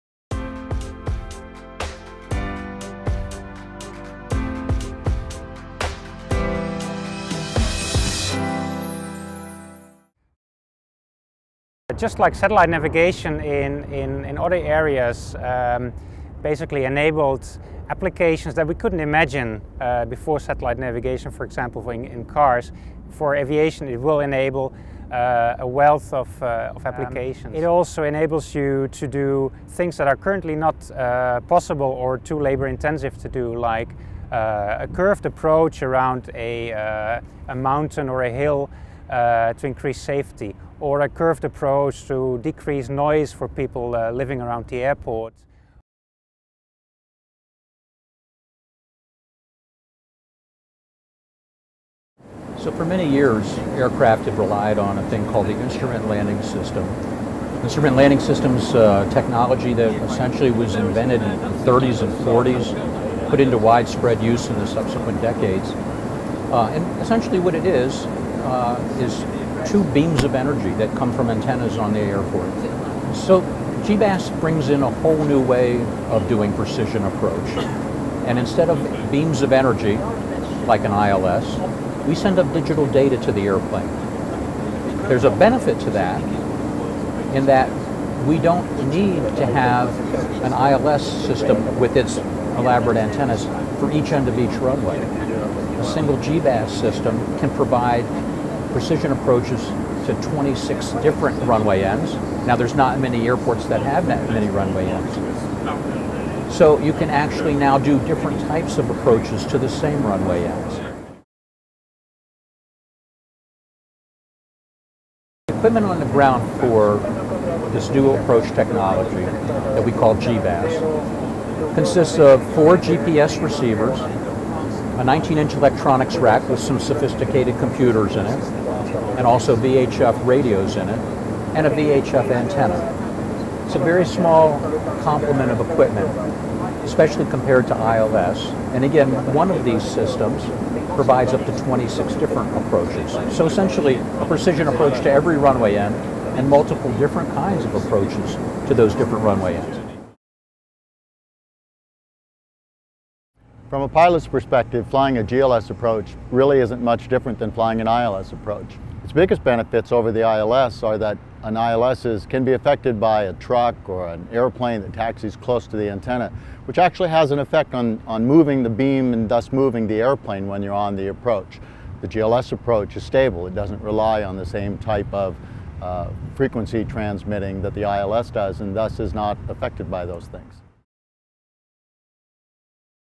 Introduction To GBAS | Products | Honeywell Aerospace
Honeywell’s flight engineers and test pilots provide an overview of Ground Based Augmentation Systems, and explain the operational benefits to operators and pilots of using satellite navigation for precision approach and landing.

Question: What Are The Benefits of Satellite Navigation?
Answer: "Just like satellite navigation in other areas has basically enabled applications that we couldn't imagine before, satellite navigation for example in cars for aviation will enable a wealth of applications. It also enables you to do things that are currently not possible or too labor-intensive to do, like a curved approach around a mountain or a hill to increase safety or a curved approach to decrease noise for people living around the airport." - Sander Roosendaal, Honeywell Aerospace

Question: How Can We Use Satellite Navigation on Approach?
Answer: "For many years, aircraft had relied on a thing called the instrument landing system (the servant landing systems technology). This technology was essentially invented in the 1930s and 1940s, and put into widespread use in the subsequent decades. Essentially, what it is is two beams of energy that come from antennas on the airport." - Pat Reines, Senior Manager at SmartPath